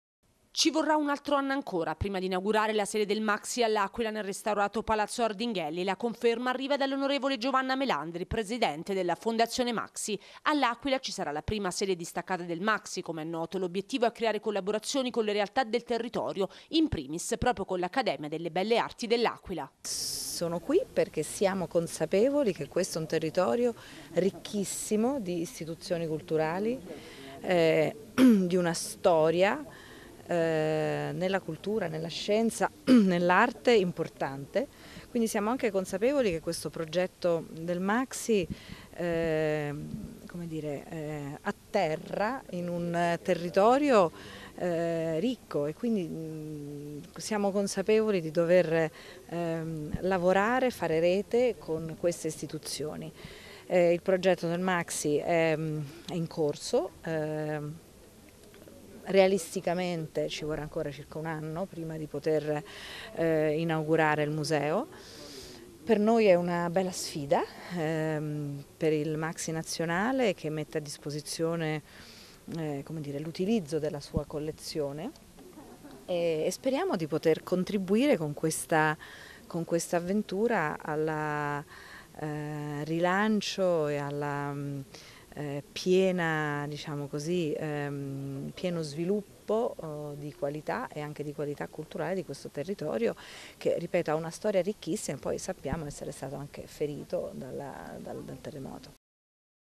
L'Aquila. Il Maxxi aprirà tra un anno
Sede distaccata del Maxxi all'Aquila. Si aprirà tra un anno assicura l'on. Giovanna Melandri. La sede, come è noto, sarà a Palazzo Ardinghelli.